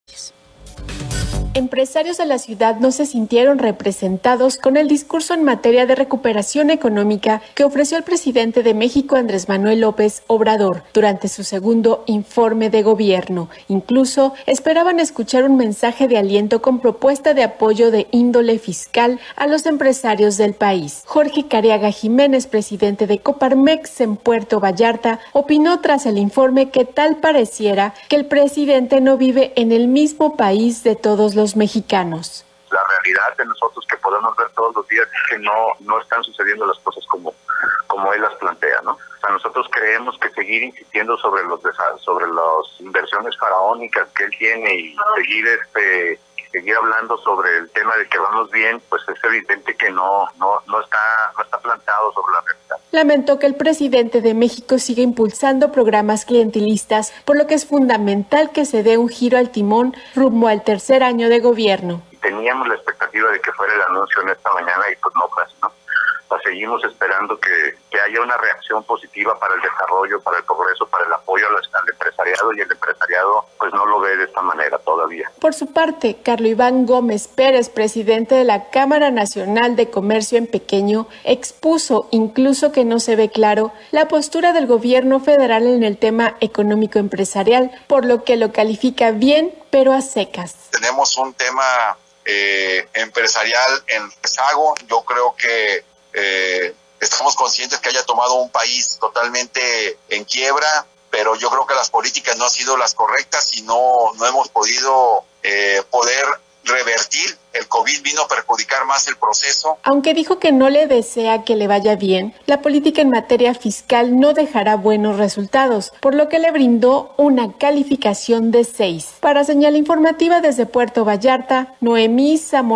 Empresarios de Puerto Vallarta no se vieron reflejados en el segundo informe
Empresarios de Puerto Vallarta no se vieron reflejados en el segundo informe de gobierno del Presidente de México.